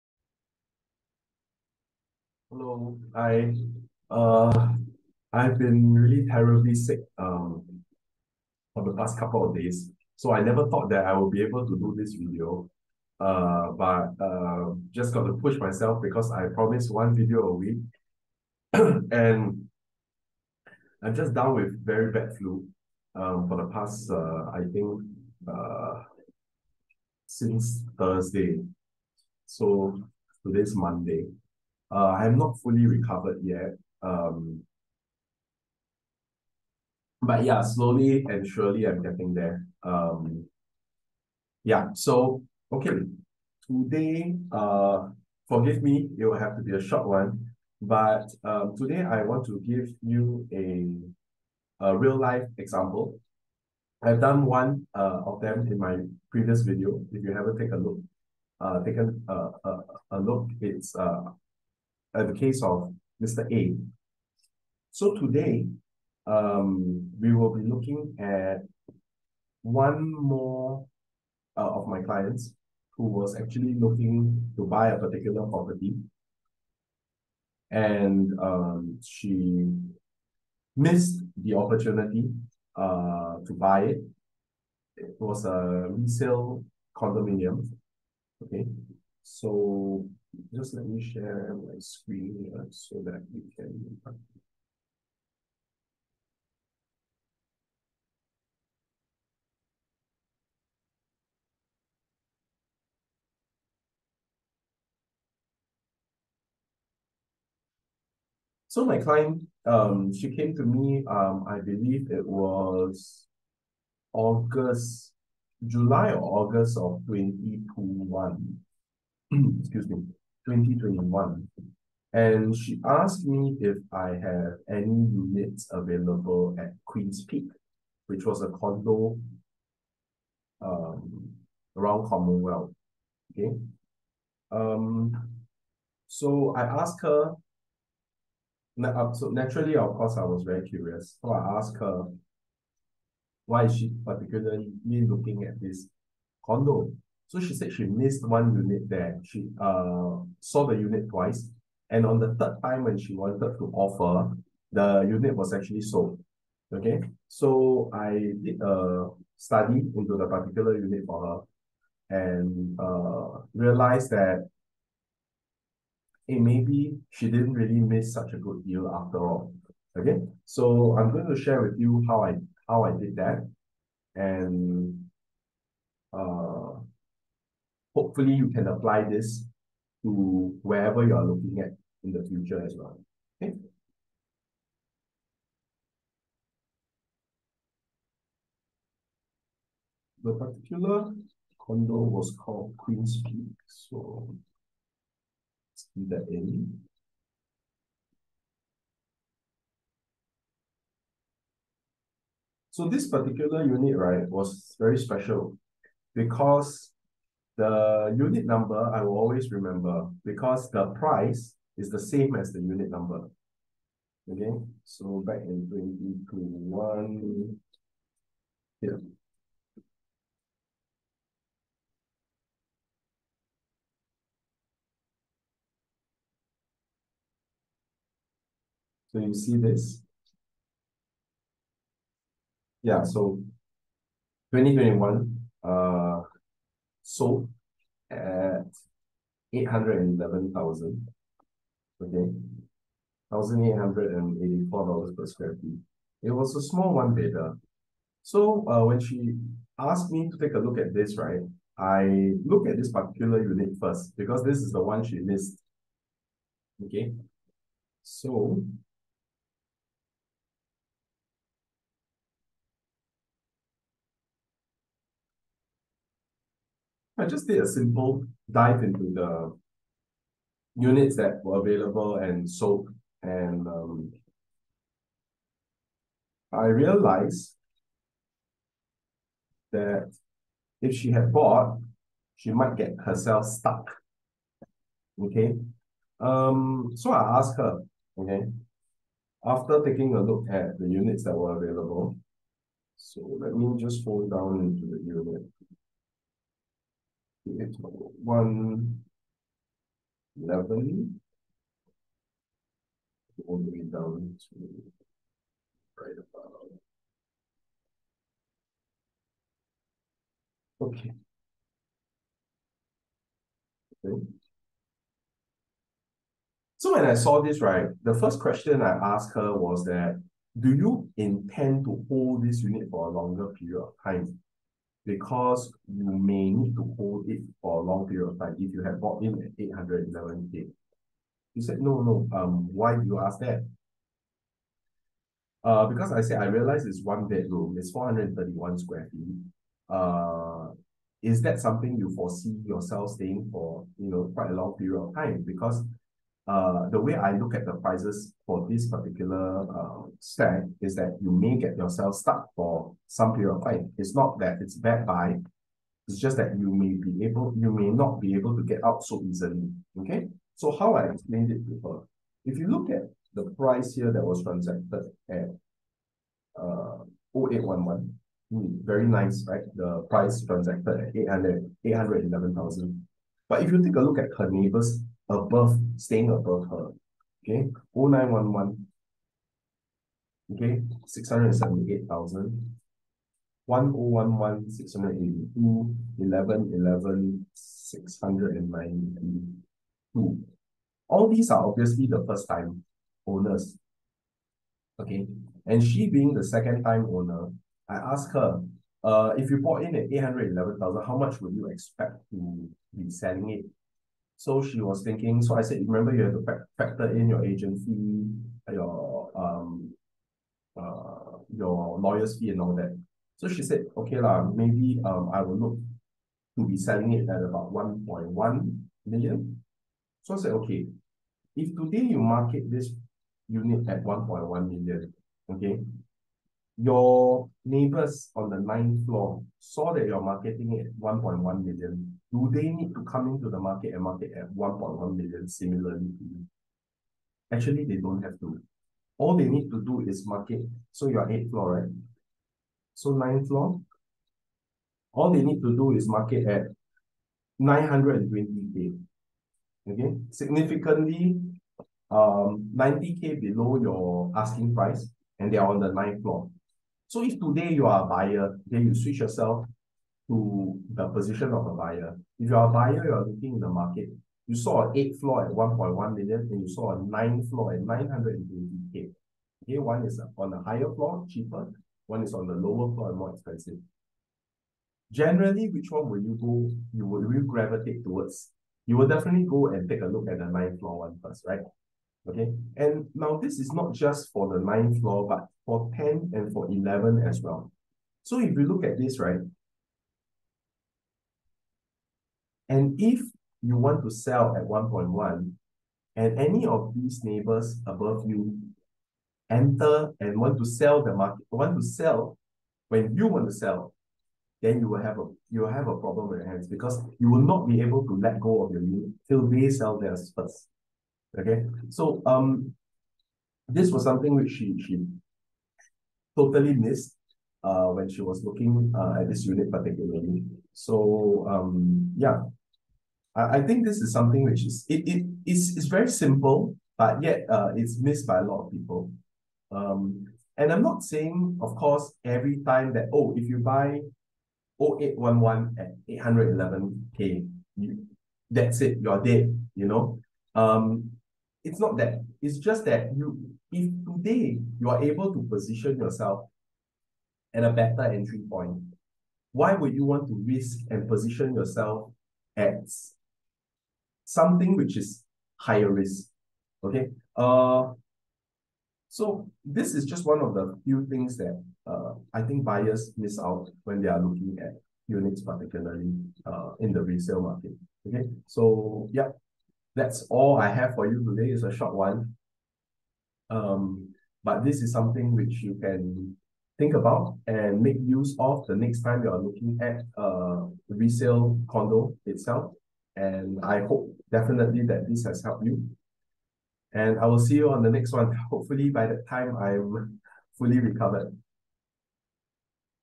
Entry price when buying a resale condo
What exactly is entry price when buying a property? How to use this to maximize your advantage when investing into real estate for the first time. More importantly what are the risks we should look out for !